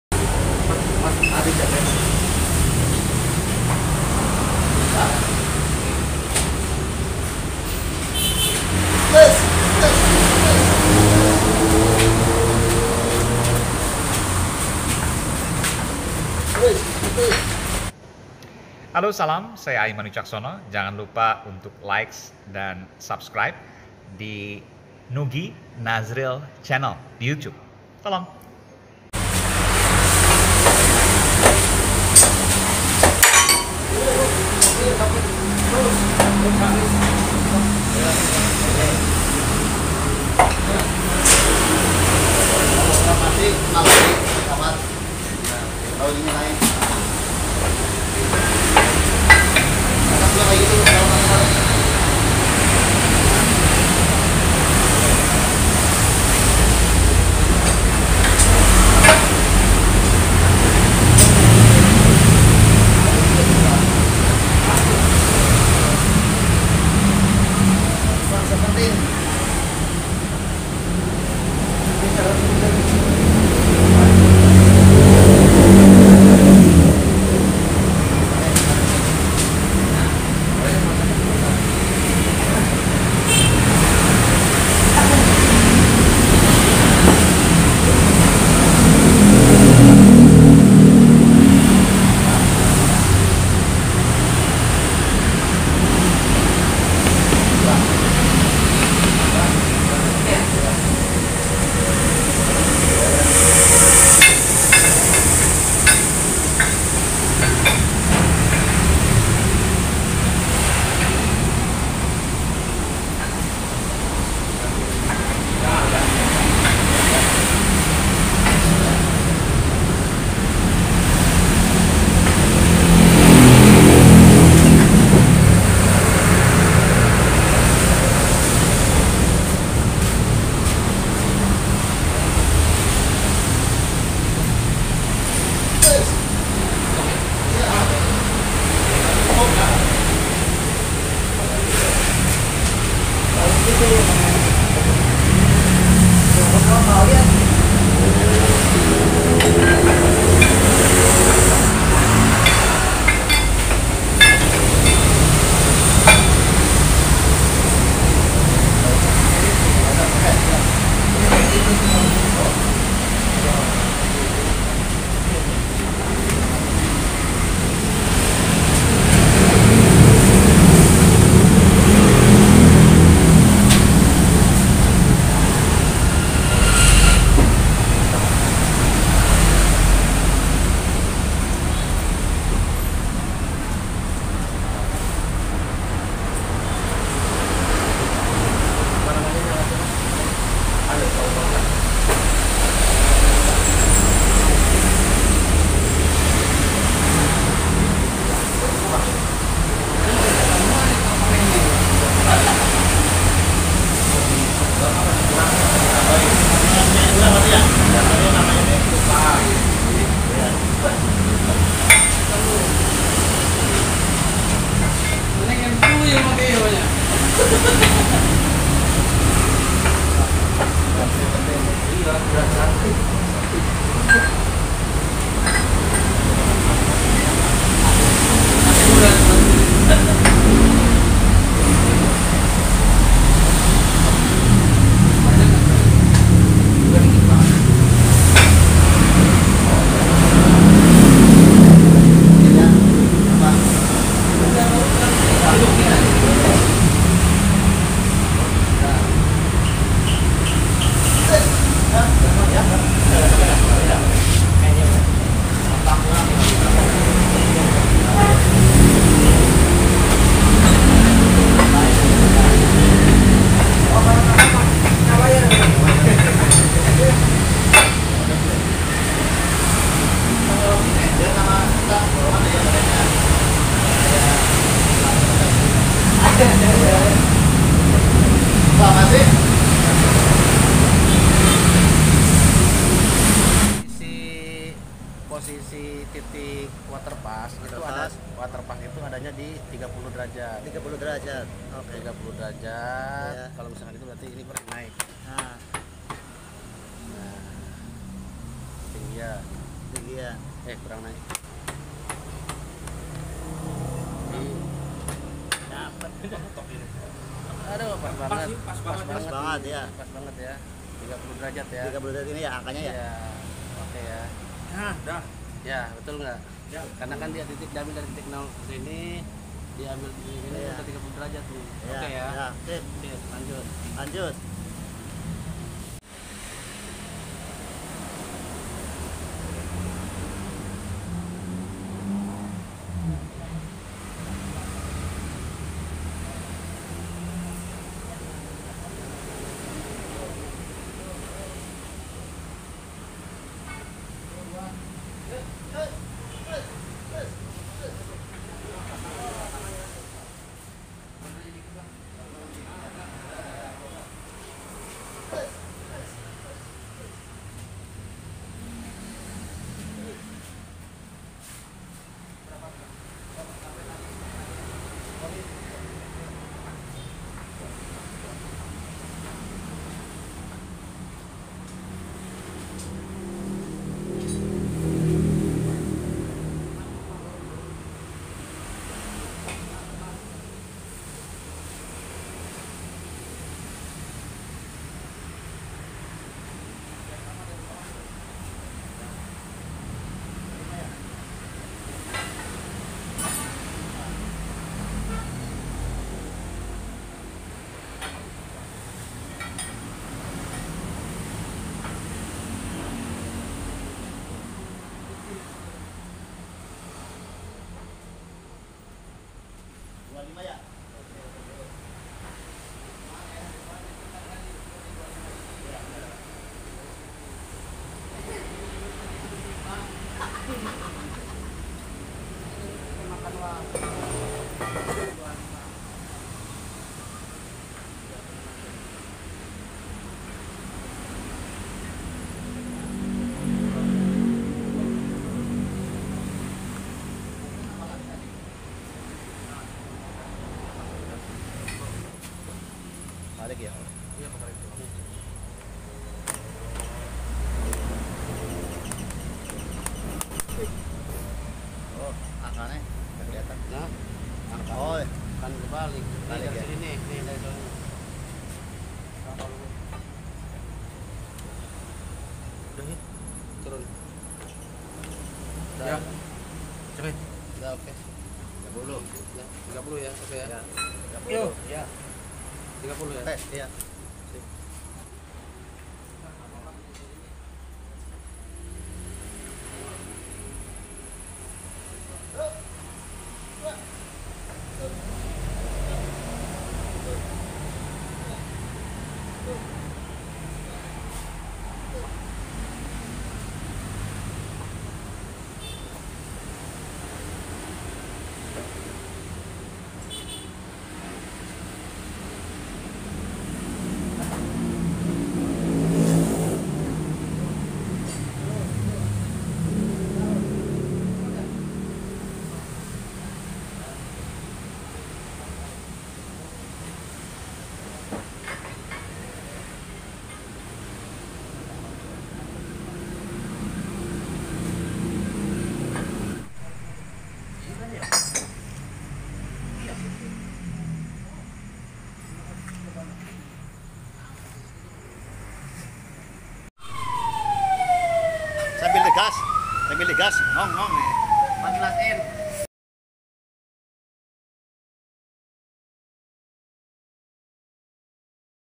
Tutorial bikin setang motor moge..
Nugie Nazril Chanel adalah Chanel yg membahas tentang otomotif unboxing review tutorial dan kali ini saya ingin berbagi informasi tentang bagaimana cara membuat stang motor Gedhe ,Harly Davidson dengan alat bending manual..semoga bermanfaat.

lokasi bengkel knalpot as tanwir jln re Martadinata no 51 Cipayung Ciputat Tangsel wa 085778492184